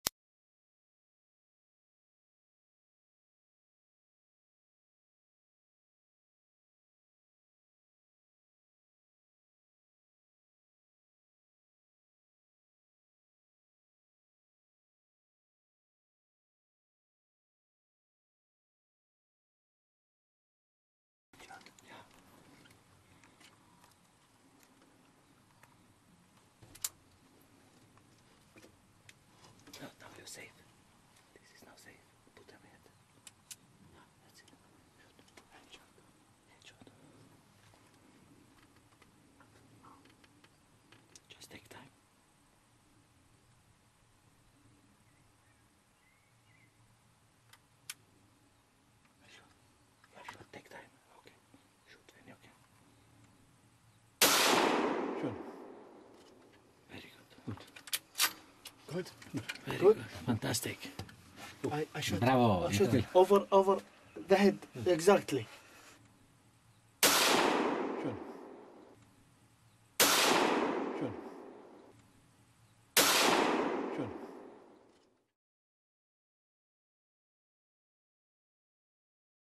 Saad Almutair
Saad Almutairi in hungaria wild bore shooting  ( HD1080p )